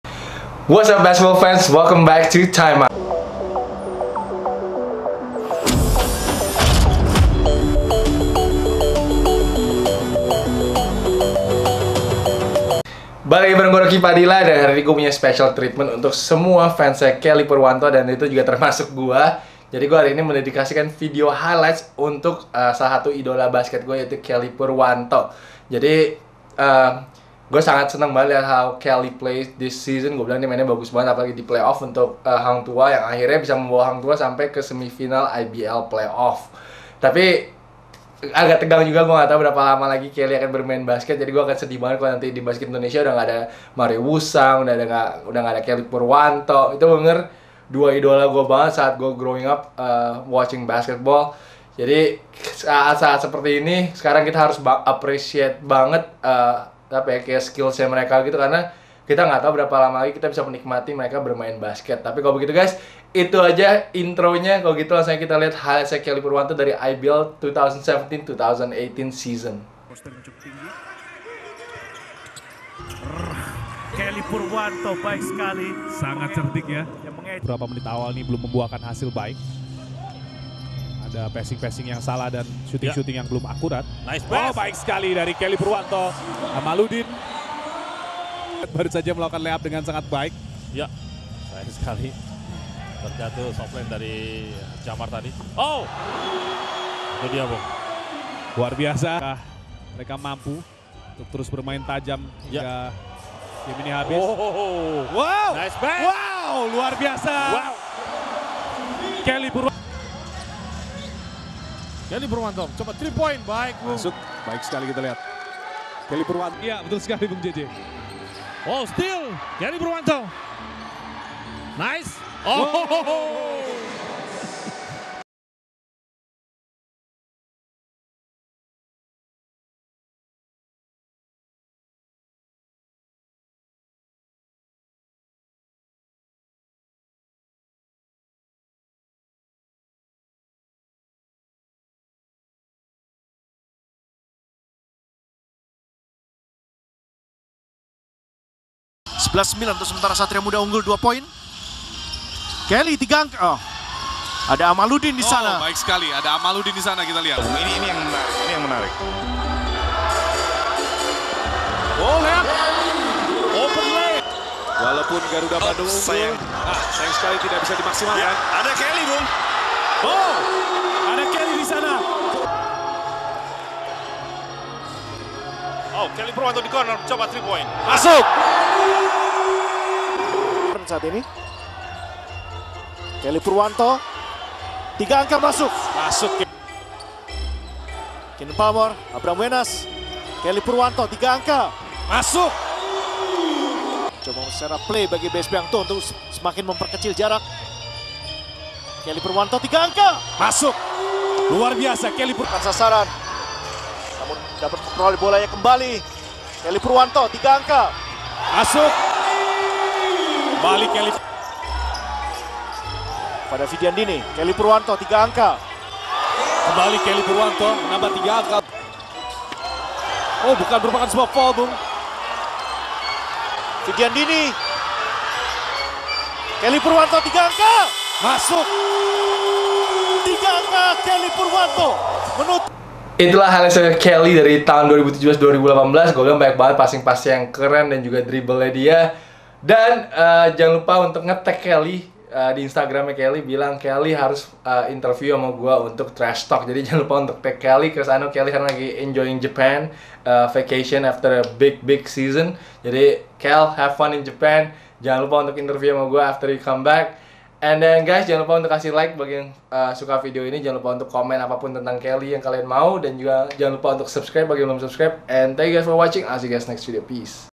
Time Out #104: Kelly Purwanto Mixtape Highlights from the 2017-2018 IBL Season!
Video specially dedicated to one of my favorite players in the IBL, Kelly Purwanto! Hope you guys will like this video.